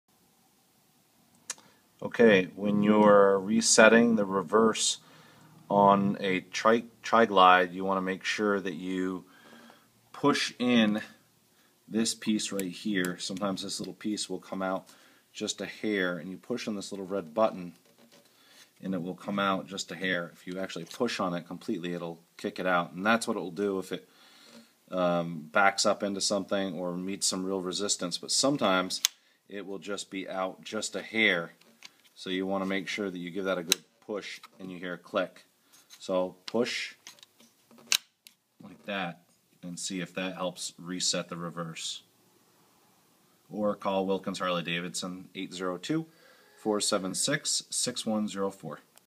Resetting reverse on a trike
The reverse gear is very sensitive on trikes.  At times the reverse fuse will kick out only so slightly.  Push in until it clicks.